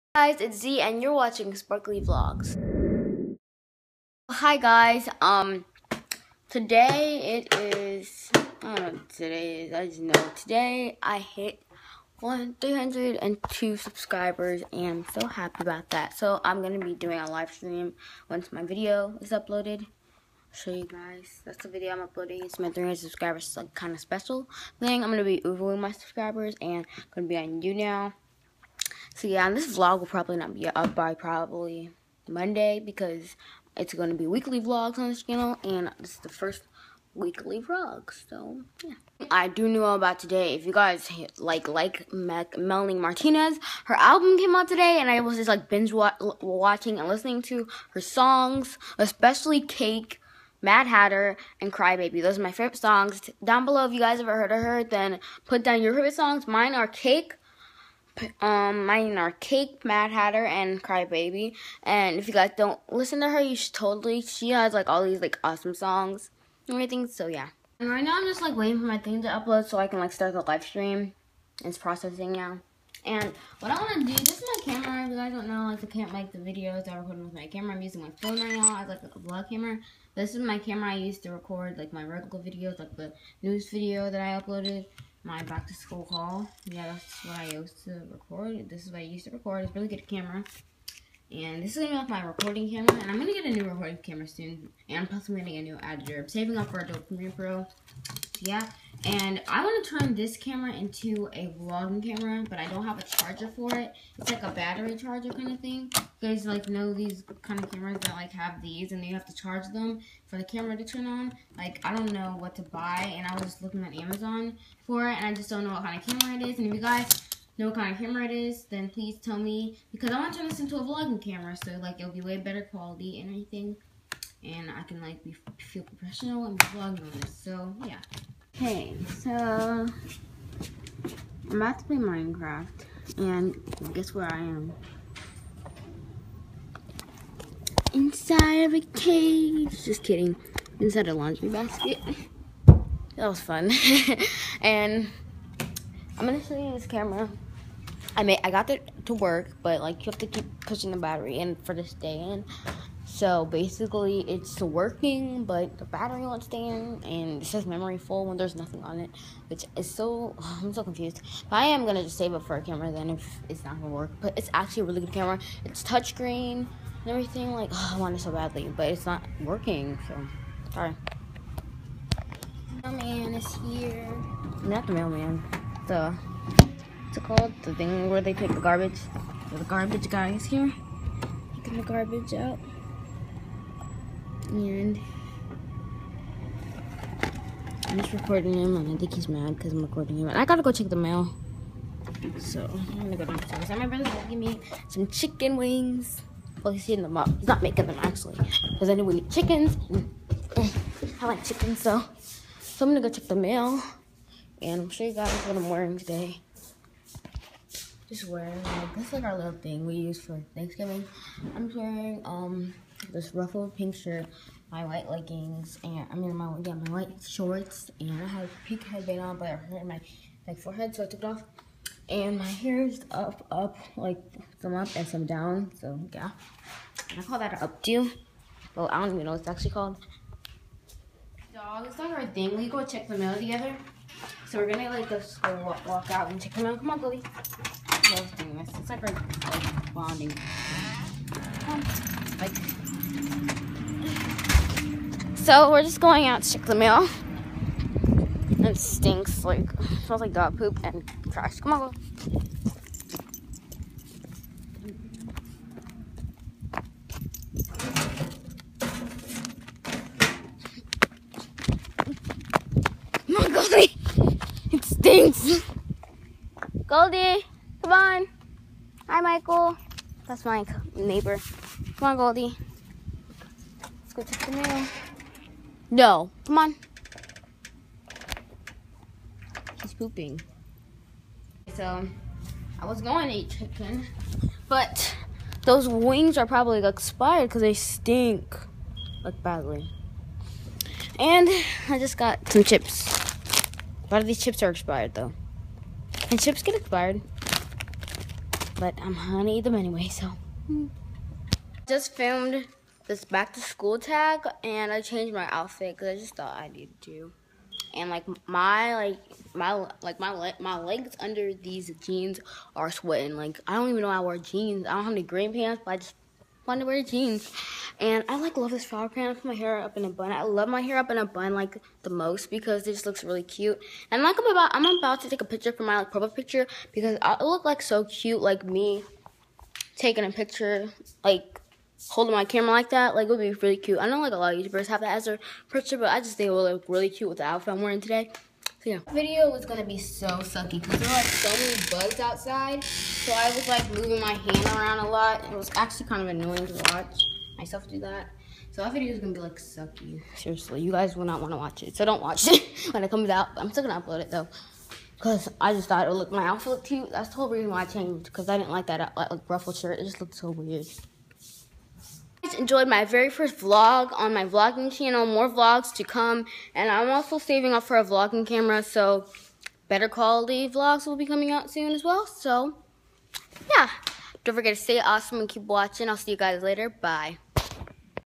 1.WELCOME TO SPARKLYVLOGS!
Hi,welcome to SPARKLYVLOGS! On this channel I will be doing challenges,vlogs, and more!! Don't forget to stay awesome and keep watching!